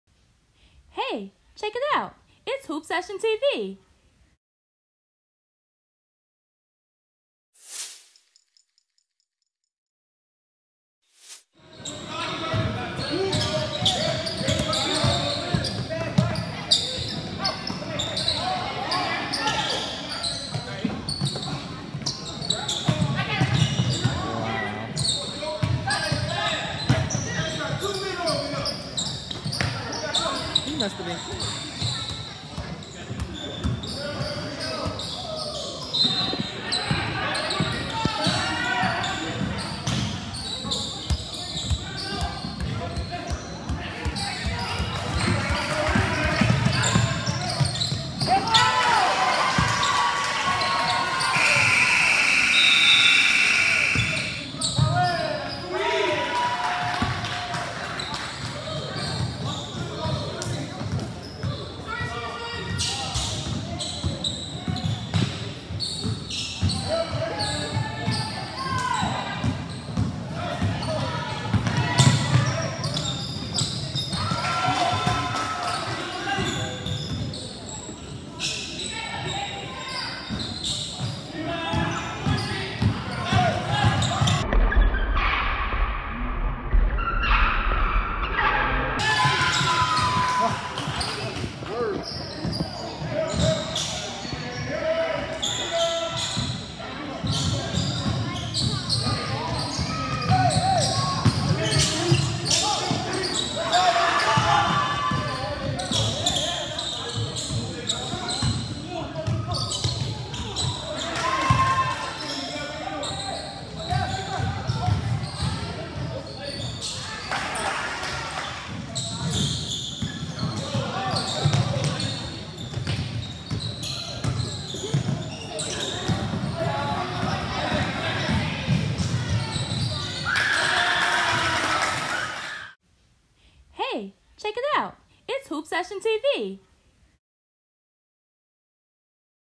Hoop Session TV: 7th Gulf Coast Warriors take on Ball Hard Elite at the Ballin on the Coast 2021
Hoop Session TV: 7th Gulf Coast Warriors take on Ball Hard Elite at the Ballin on the Coast 2021. Last pool play game Saturday night and Gulf Coast Warriors game out running! Ball Hard Elite never got out the gate. The Warriors got steals, multiple rebounds, and just had their way. The energy wasn’t there for Ball Hard Elite Saturday night. Regroup and keep getting better. The Warriors were hot heading into bracket play. Check out the full game highlights on YouTube.